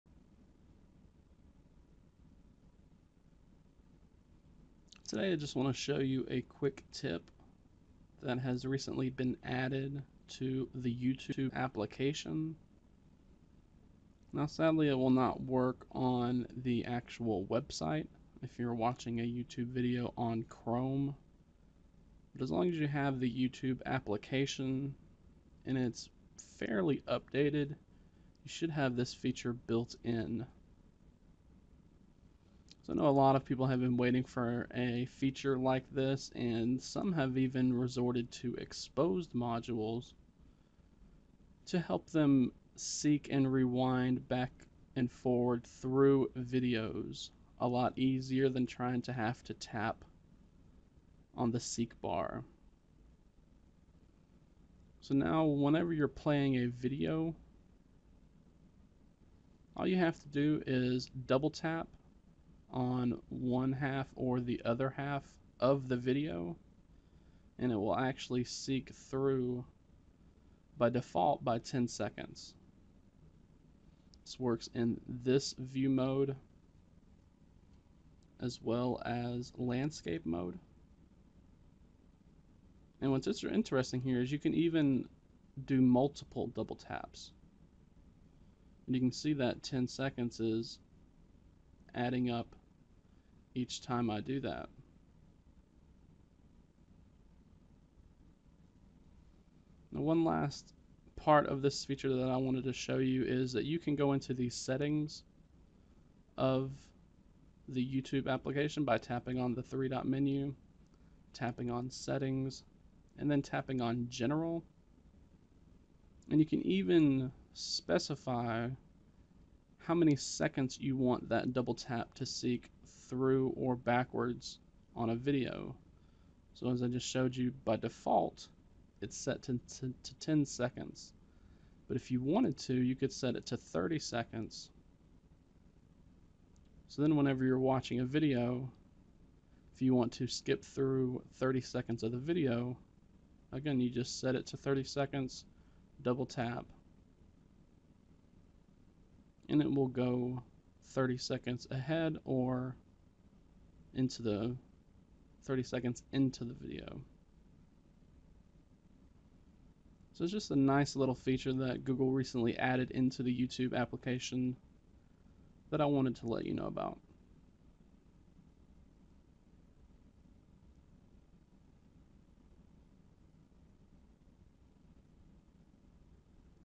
[YouTube] Using the Double Tap to Seek Gesture
If you have a relatively updated version of YouTube, you should know that there's a double tap to seek gesture built right into it.